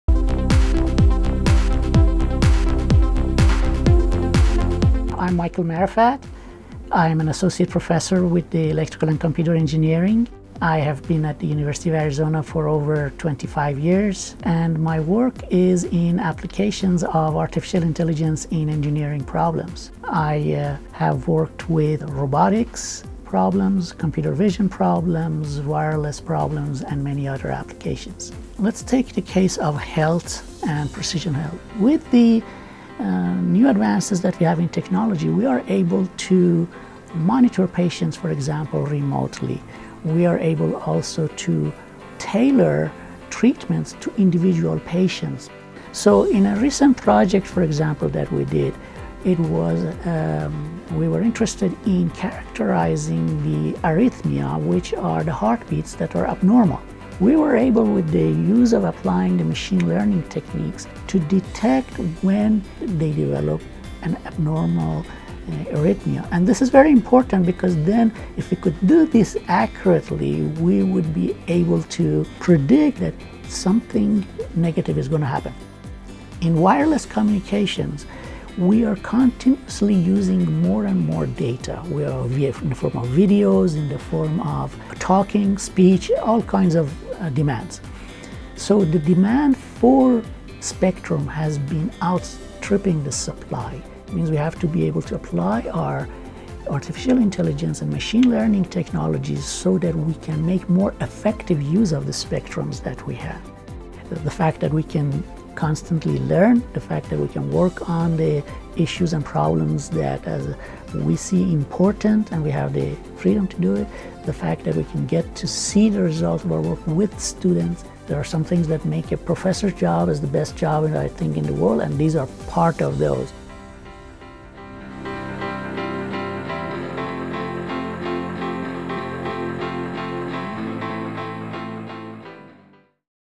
Michael Marefat: ECE Faculty Profile
Michael Marefat is the associate professor in the Electrical and Computer Engineering program at the University of Arizona. His expertise is in artificial intelligent systems, computer vision and robotics, machine learning, software engineering, and biomedical applications.